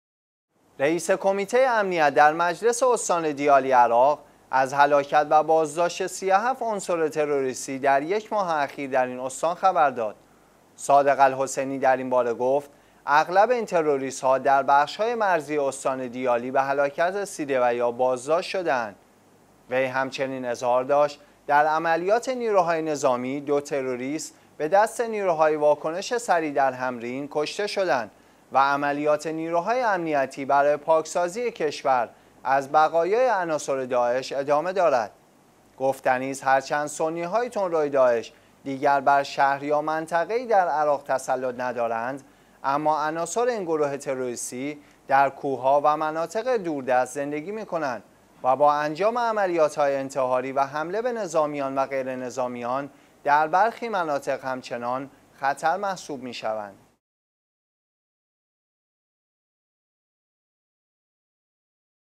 بازداشت و هلاکت ۳۷ تروریست در استان دیالی عراق
رئیس کمیته امنیت در مجلس استان دیالی عراق از هلاکت و بازداشت 37 عنصر تروریستی در یک ماه اخیر در این استان خبر داد.
«صادق الحسینی»  در این باره، گفت: اغلب این تروریست ها در بخش های مرزی استان دیالی به هلاکت رسیده و یا بازداشت شده اند.
وی همچنین اظهار داشت: در عملیات نیروهای نظامی دو تروریست به دست نیروهای واکنش سریع در حمرین کشته شدند و عملیات نیروهای امنیتی برای پاکسازی کشور از بقایای عناصر داعش ادامه دارد.
گفتنی است هرچند سنی های تندروی داعش دیگر بر شهر یا منطقه ای در عراق تسلط ندارد اما عناصر این گروه تروریستی در کوه ها و مناطق دوردست زندگی می کنند و با انجام عملیات های انتحاری و حمله به نظامیان و غیرنظامیان در برخی مناطق همچنان خطر محسوب می شوند.